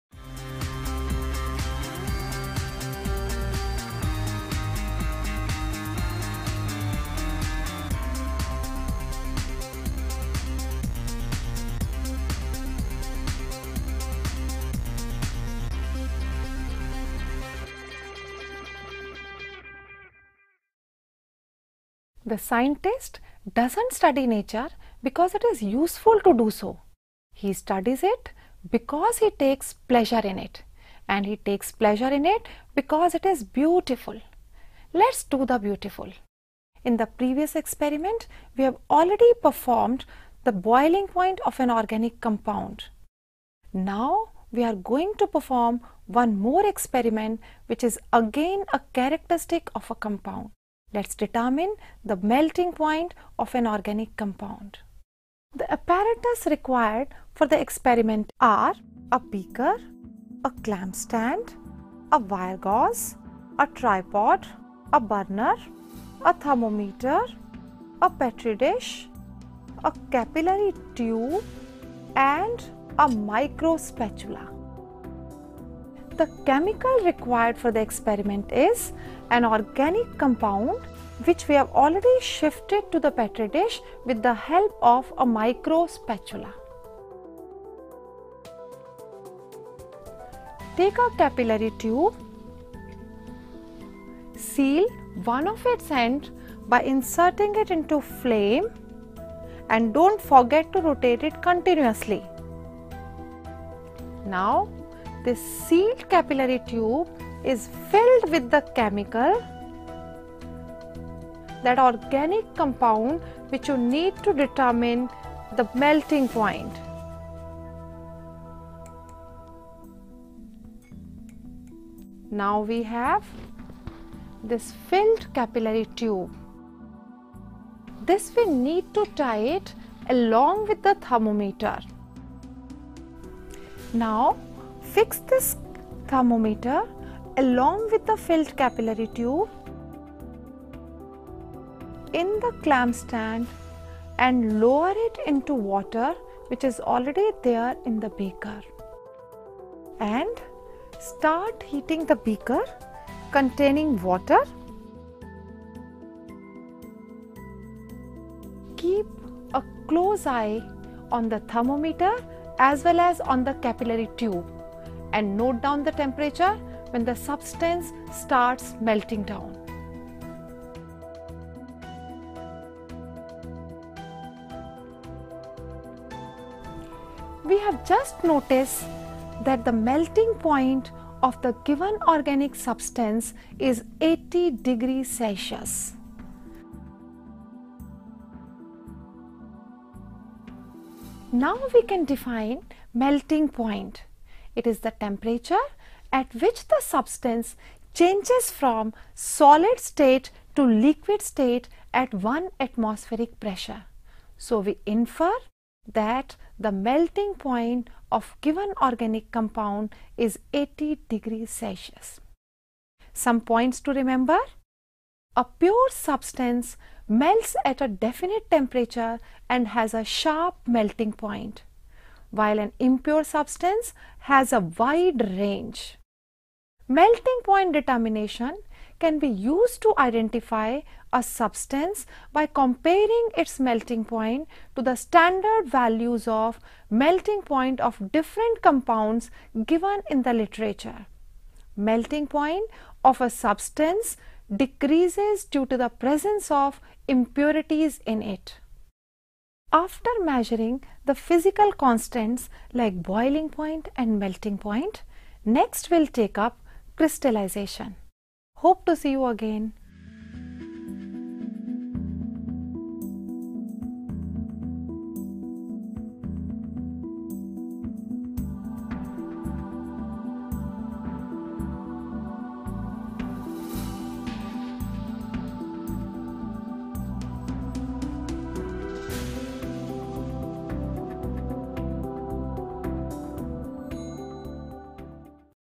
Chemistry Lab Kit Experiment 02
Seeing is believing. It is the main theme of science experiments. This experiment shows the melting point of an organic liquid.